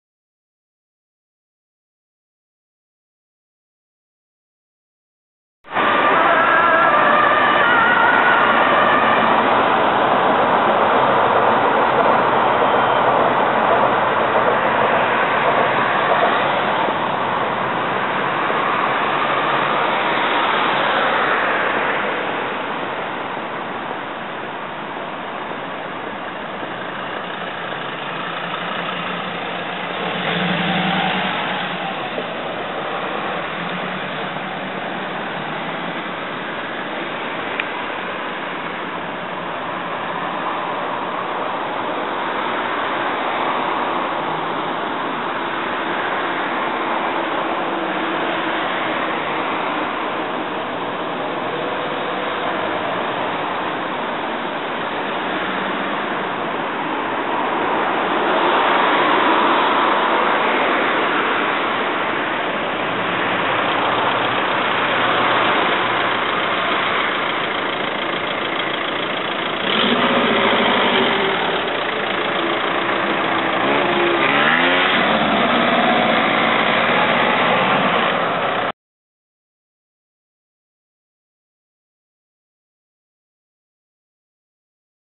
A1 AMBU 16-128 ALBINUSDREEF LEIDEN aankomst A1 RPZ Spijkenisse
Ambu komt met A1 aan in RPZ Spijkenisse